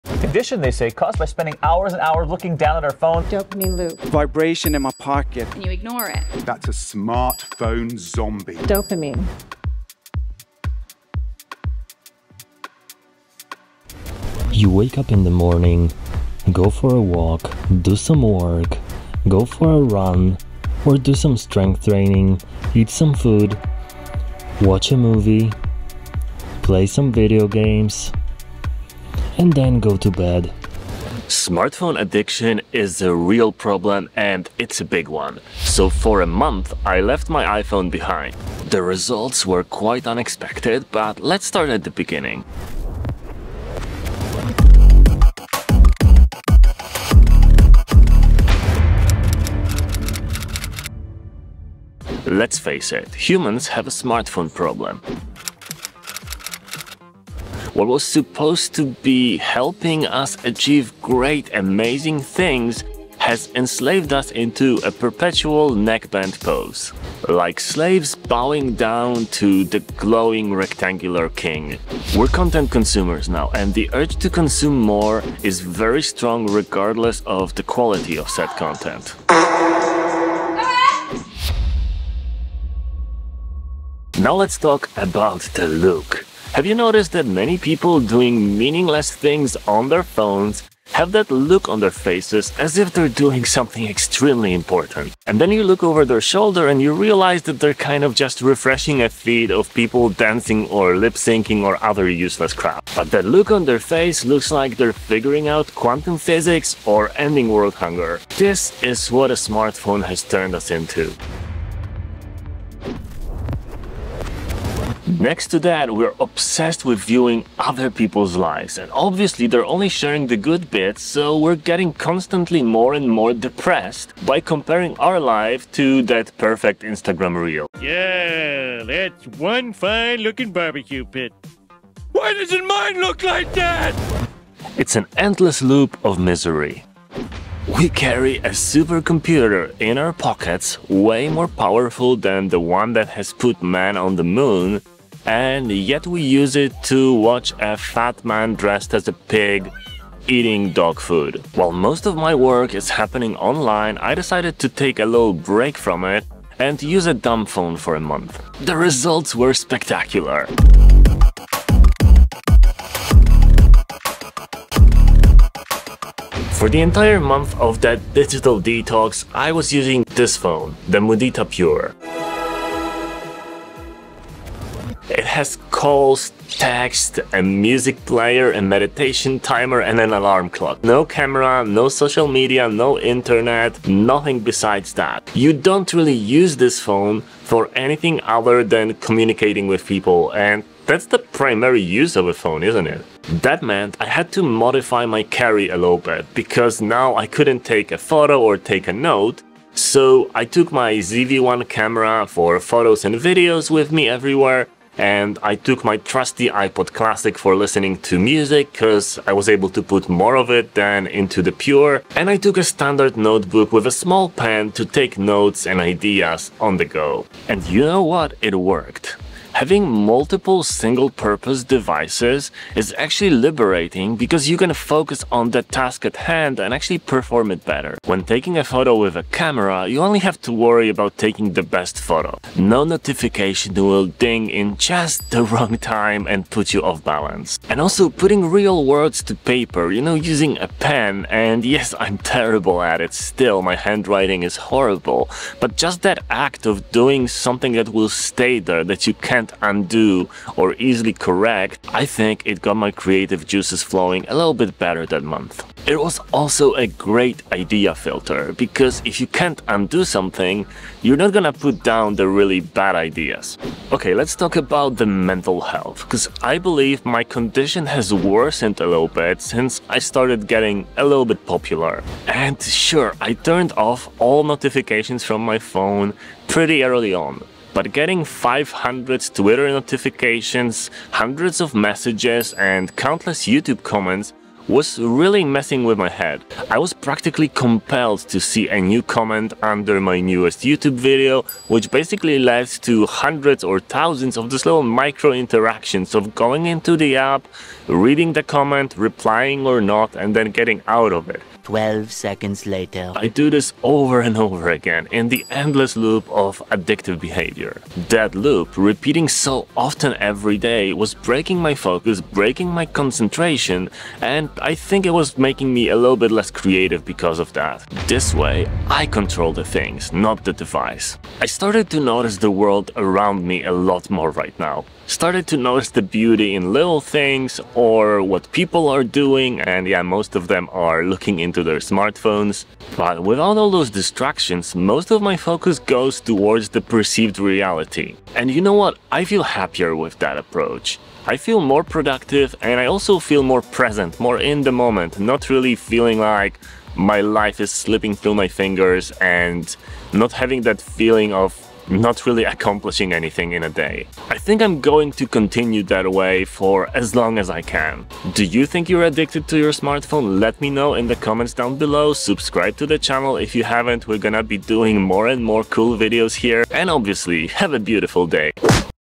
Are you a Smartphone Zombie?
Smartphone Zombies (smombies) is a new addiction that is happening on a massive scale all over the globe. Next to depression, anxiety and other mental issues, we also have increasing neck strain, back muscle atrophy, eye-strain, lower sleep quality and a lot more.

And we are doing this to ourselves through a dopamine loop delivered by that innocent looking, powerful device in our pockets.

Digital detox, or a dopamine detox are a good way to reset yourself and your connection to your devices. You may start seeing the real world™ again and it can look really beautiful!